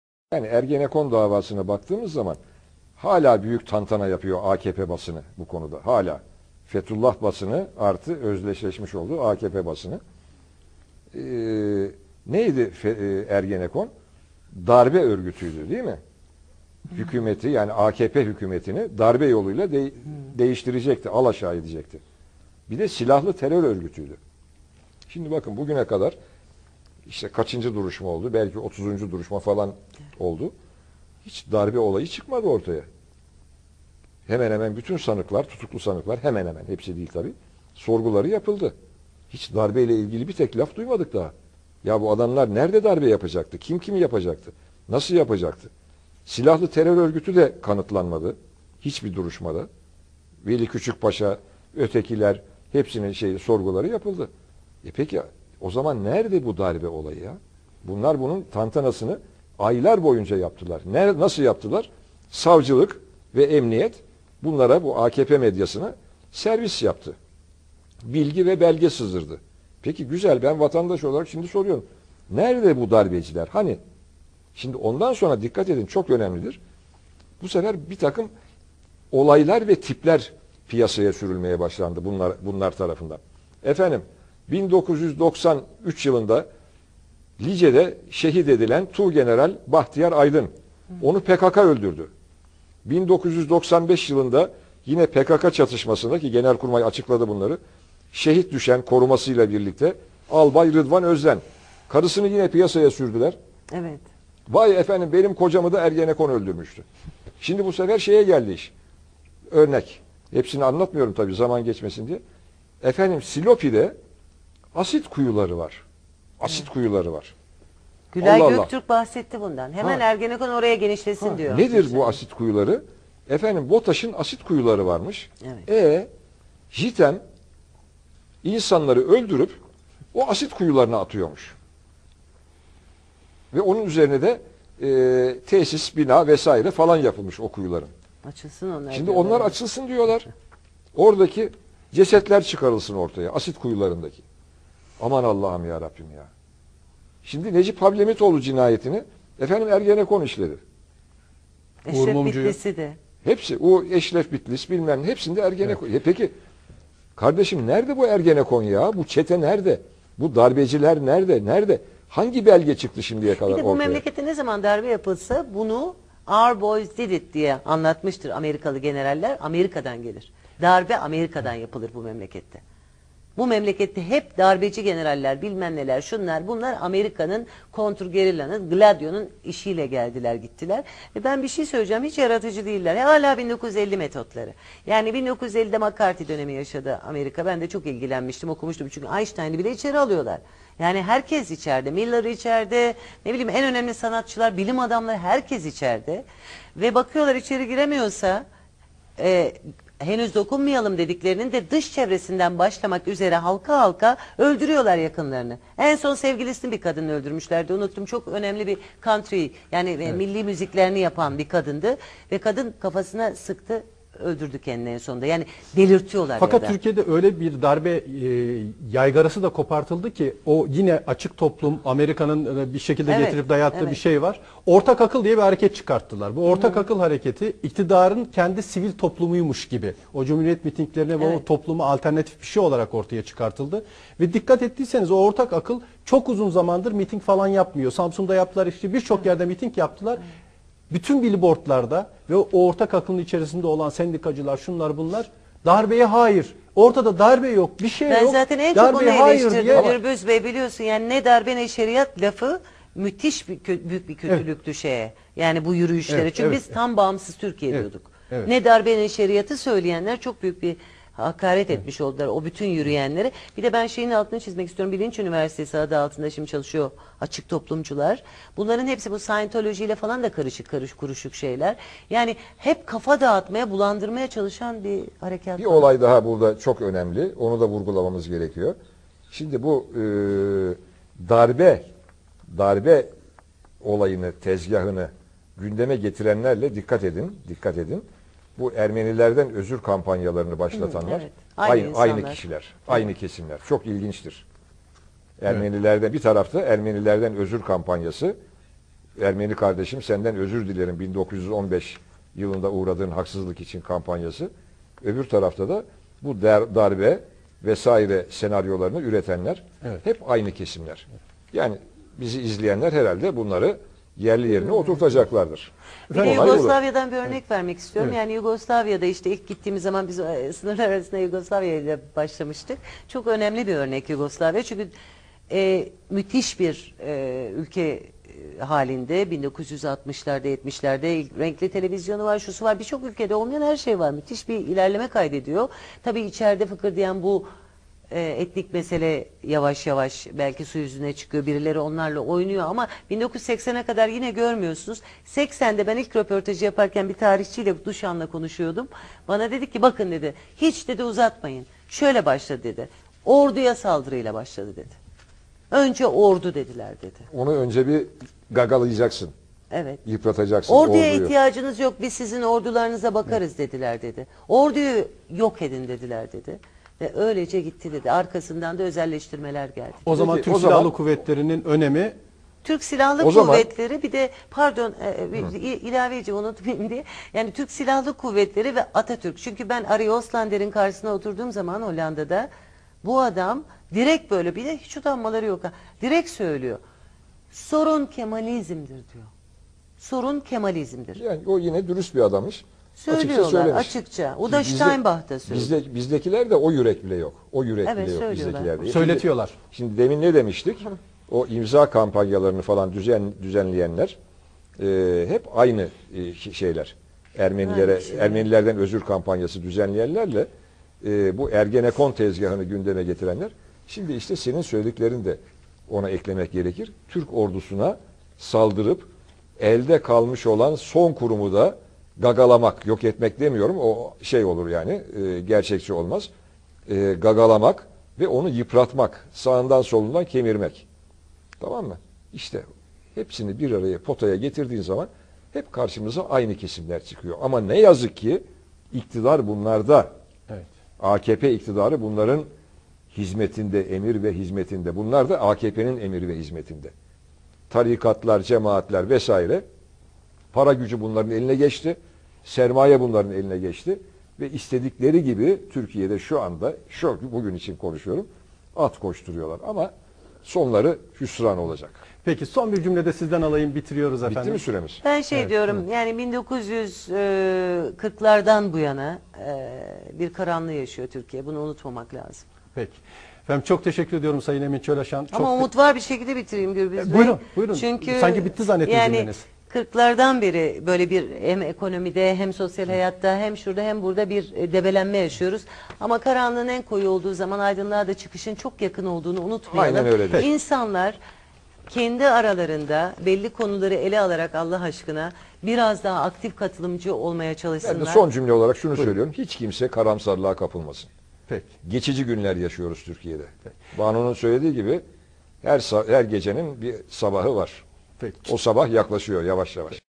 Ermenilerden Özür Dileyen AKP yandası Medya ve Ergenekon - Emin Çölaşan Banu Avar
sözde ergenekon tertibini hazırlayan sözde soykırımın destekcisi AKP medyası